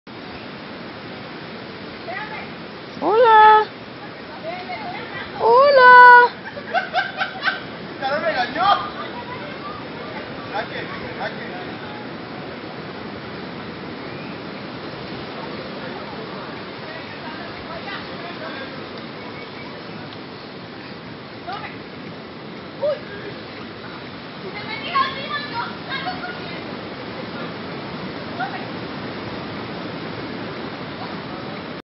Mono Cara Blanca Costa Rica
El mono carablanca, maicero cariblanco, capuchino, tanque, machín, caurara o carita blanca2​ (Cebus capucinus) es un mono del nuevo mundo de tamaño medio perteneciente a la familia Cebidae.

Es nativo de los bosques de América Central y de la parte más noroeste de Sudamérica y muy valioso por su papel como dispersor de semillas y polen. En los últimos años se ha convertido en una especie muy popular en Norteamérica.